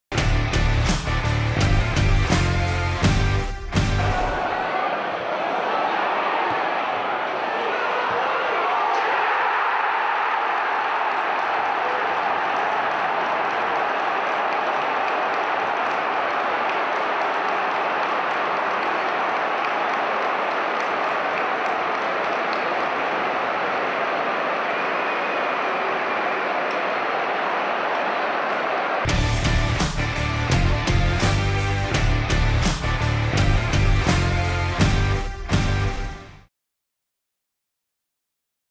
GoalCam: David McGoldrick's opener
Through the net footage of David McGoldrick's opening goal at the DW Stadium.